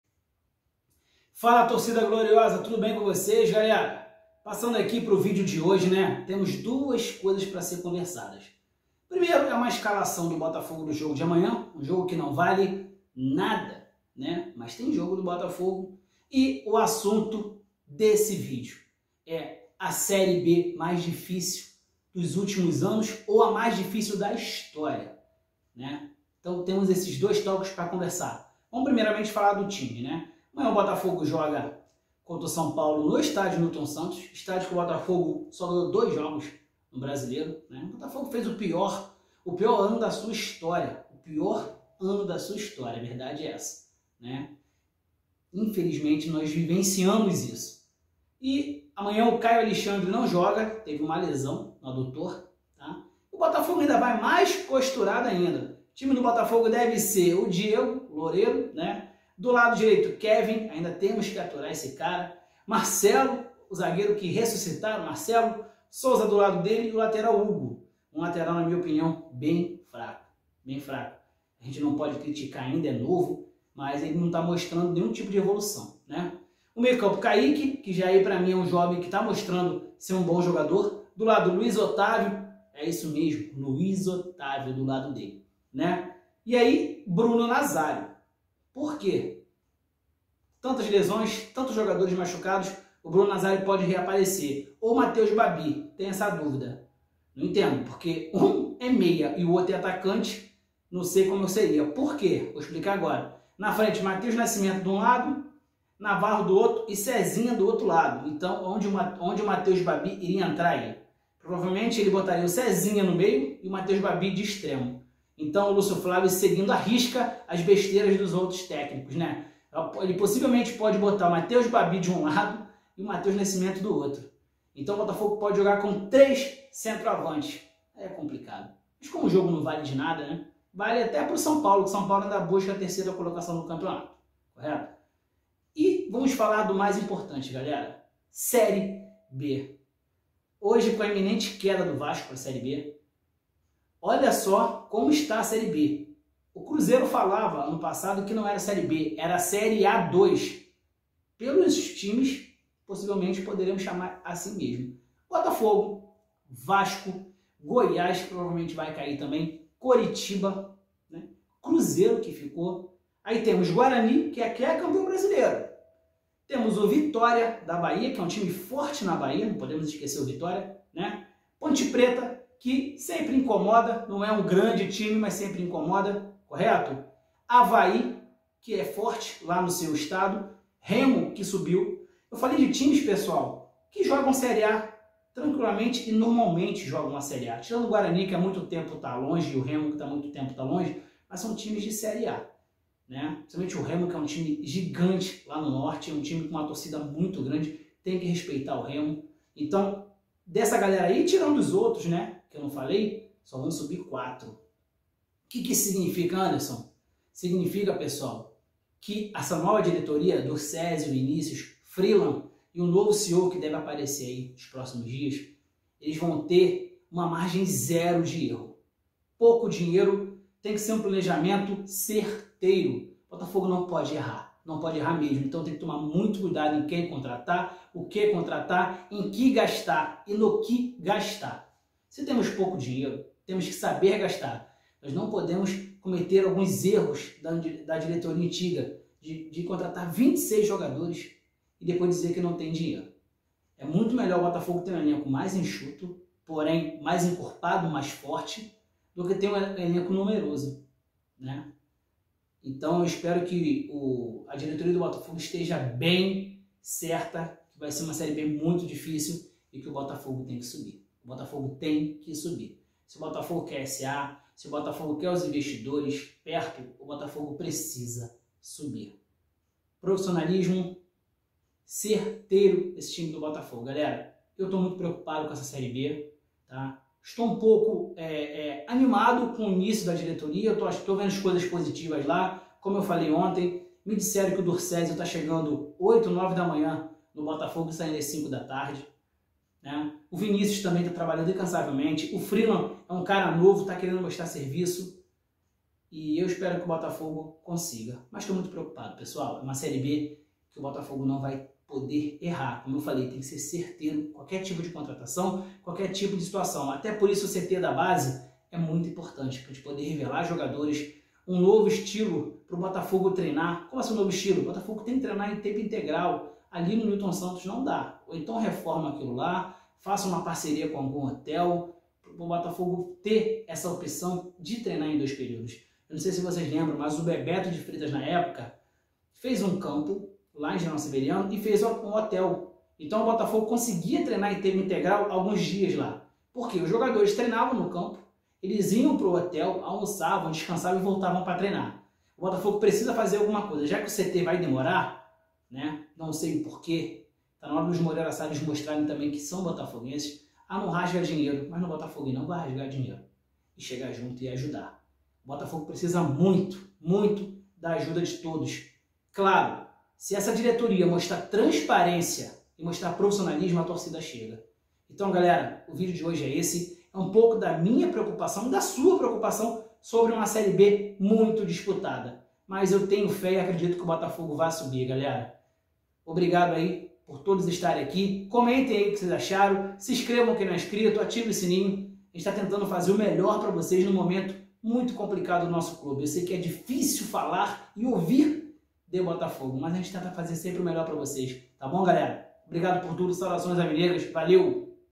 ⚠️CONFIRMADO: SÉRIE B MAIS DIFÍCIL DA HISTÓRIA TERÁ BOTAFOGO, CRUZEIRO E VASCO, GIGANTES DO FUTEBOL
O Vasco da Gama caiu. Dessa forma se junta ao Botafogo e ao Cruzeiro na Série B mais difícil de todos os tempos. Com os três gigantes na segunda divisão, são doze títulos nacionais de série A (somando os títulos de Coxa e Guarani), e ainda pode acontecer de cair o Bahia, com dois canecos nacionais.